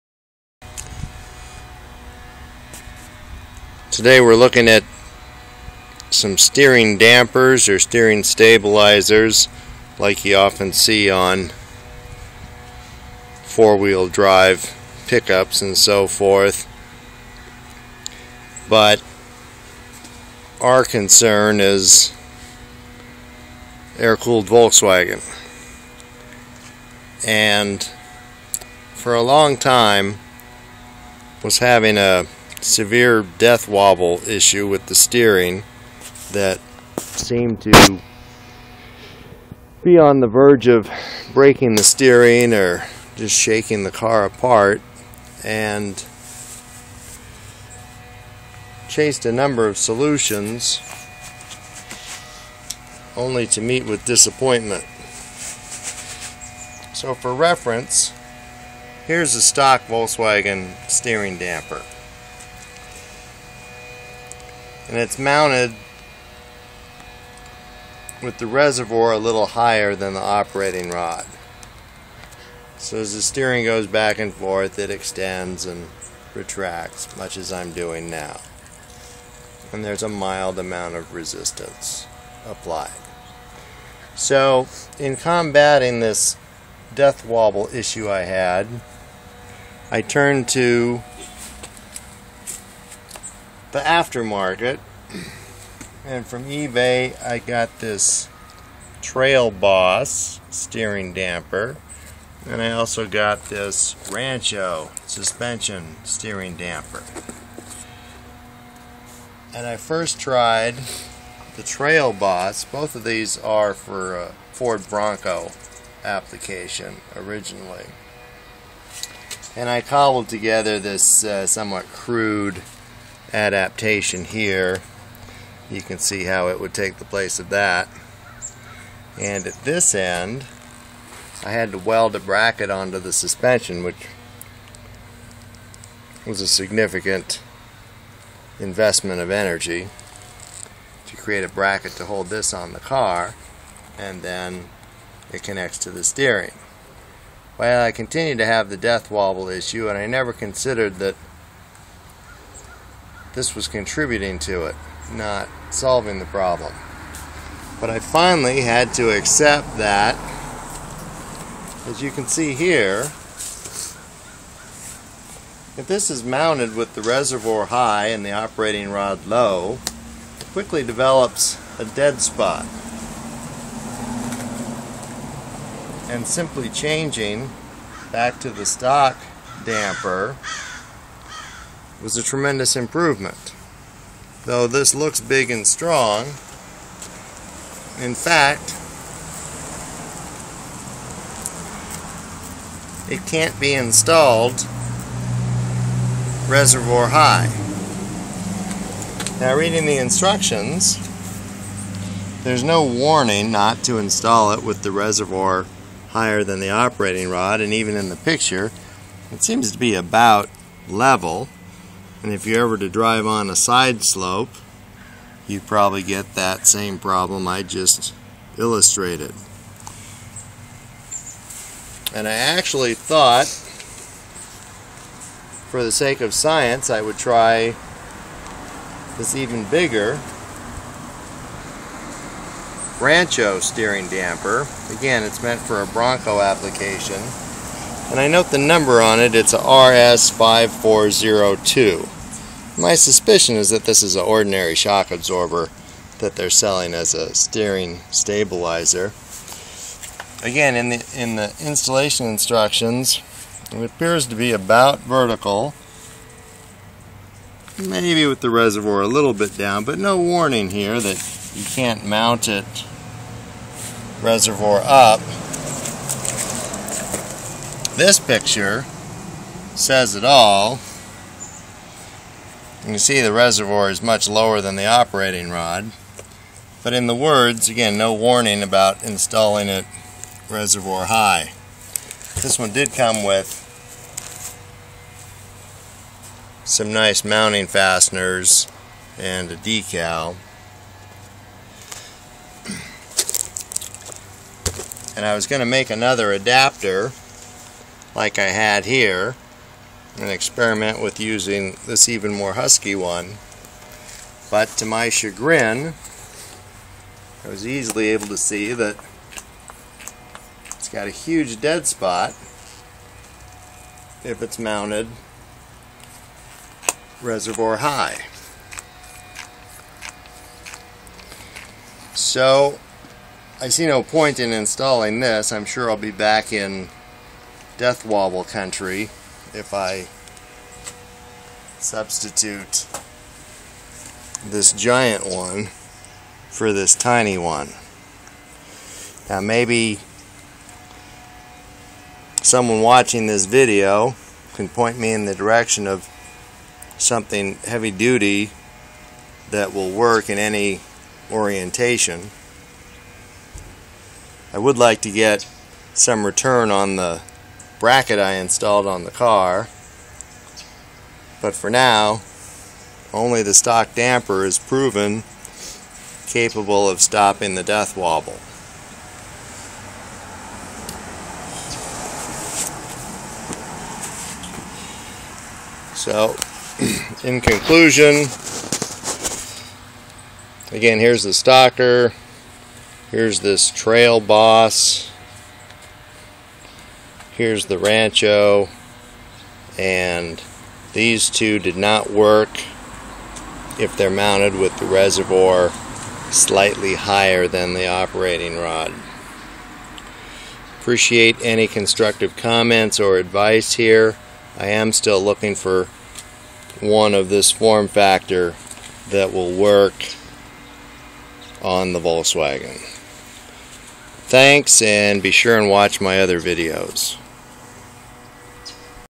These Steering Dampers Didn't Work on VW Baja Bug MOV126.mp4
Both of these steering dampers will suck air and stop providing damping if the reservoir is held or mounted higher than the operating rod.  Chased a death wobble problem for months before I had to accept that the "Trail Boss" simply didn't work installed at a slight angle. Tried the "Rancho", just flexing it back and forth by hand, it also stops working if the reservoir is higher than the operating rod.  These are both meant for a Ford Bronco ... now looking for one that doesn't depend on gravity. ( at 4:15 I say "vertical" but obviously meant "horizontal" )